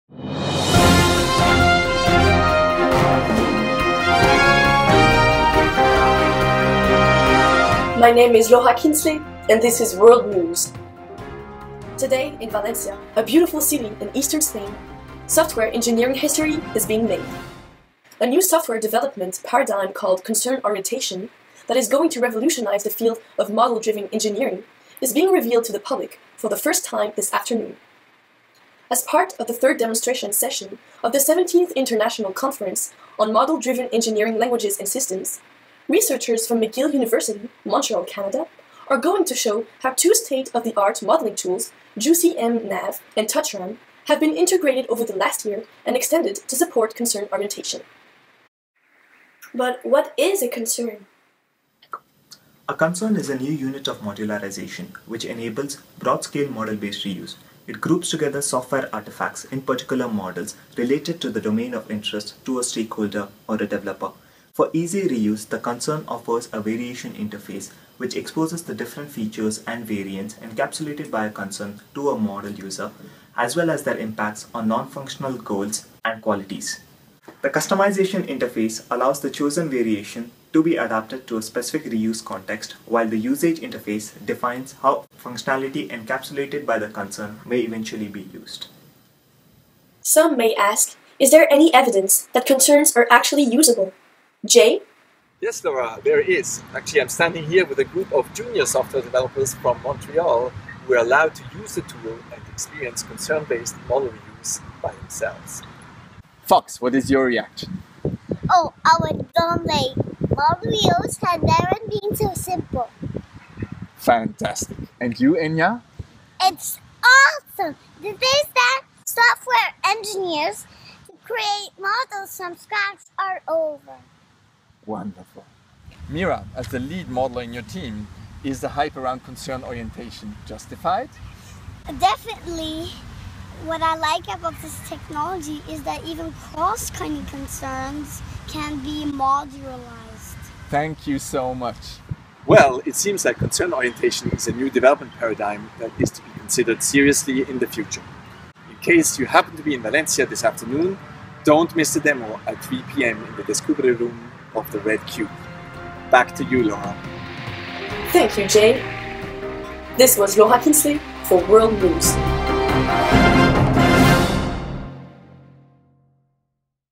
TouchCORE: Concern-Oriented Software Development - MODELS 2014 Demo Teaser
This video was created as a demonstration teaser video for the MODELS 2014 conference.

Actors (in order of appearance):
Laura Kienzle
Omar Alam
Matthias Schöttle
Nishanth Thimmegowda
Jörg Kienzle
Fox Kienzle
Enya Kienzle
Mira Kienzle